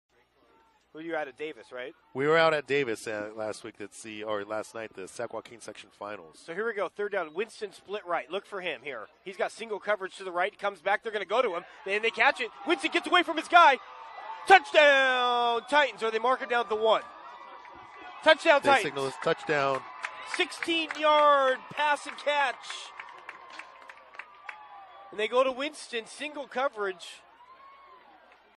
#2 Tyler Winston of Antelope breaks a tackle and scores from 16 yards out
Tyler Winston of the Antelope Titans scores after breaking a tackle from 16 yards out against the Buhach Colony Thunder in this CIF Sac-Joaquin Section Division II Quarter Final match up.